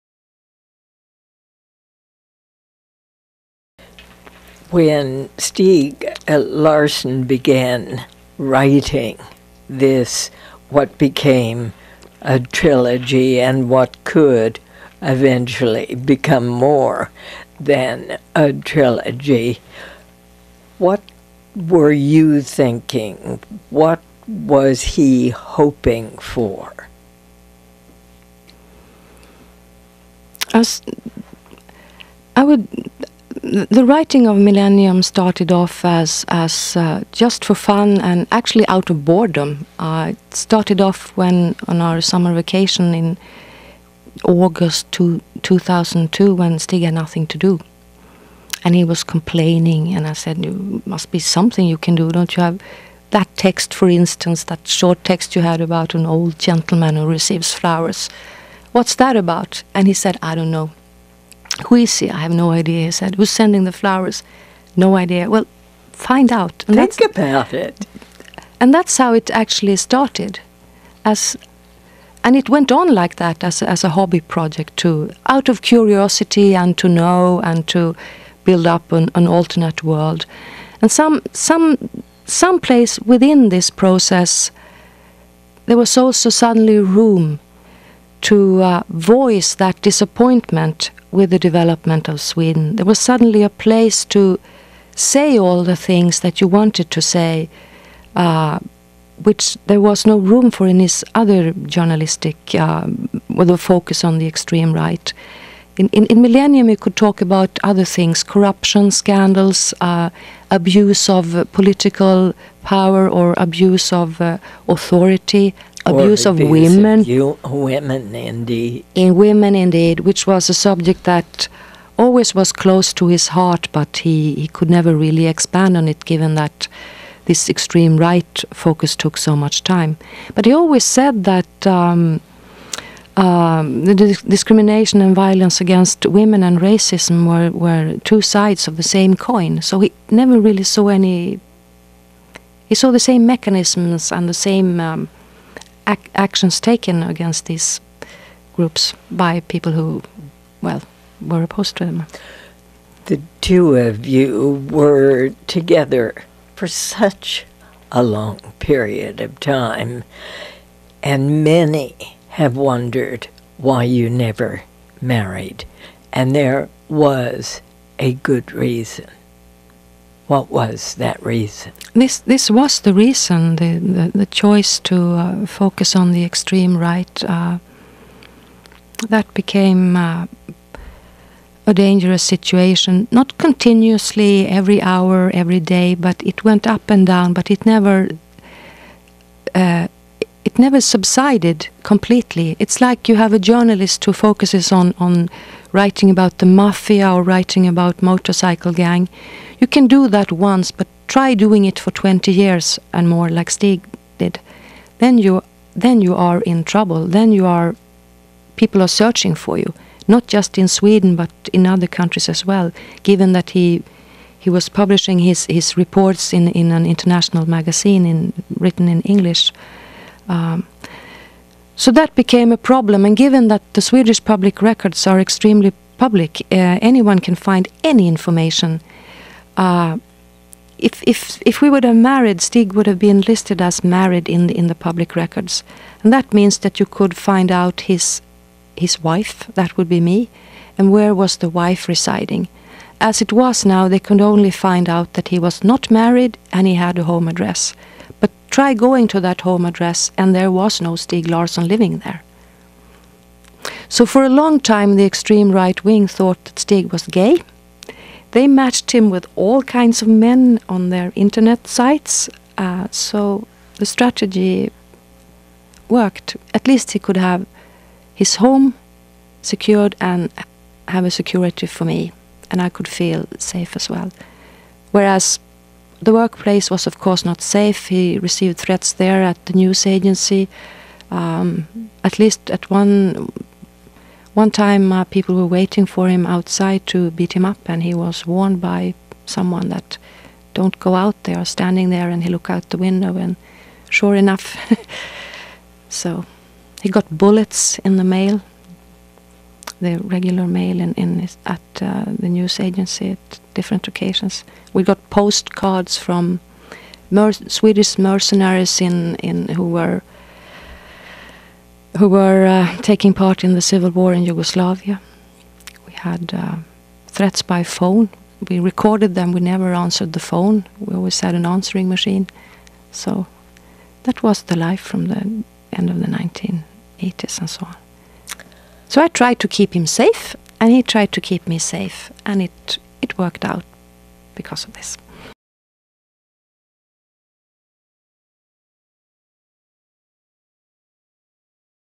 Gabrielsson on the Genesis of Stieg Larsson's Millennium Trilogy
The Diane Rehm Author, architect, and political activist Eva Gabrielsson talks about her relationship with Millennium Trilogy author Stieg Larsson and the genesis of the books.